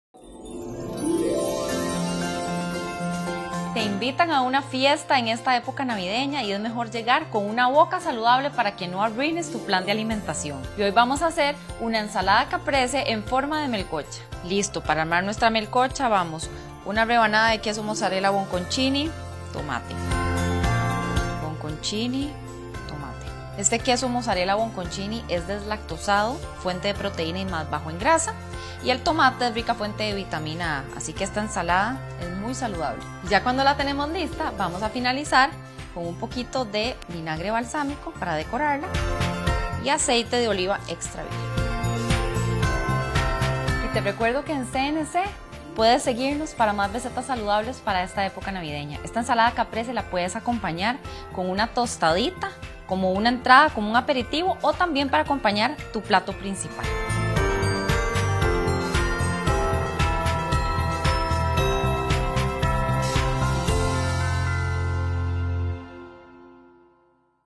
Melcochas de Navidad
La versión de Ensalada Caprese en forma de melcocha para celebrar con amigos y familia la Navidad.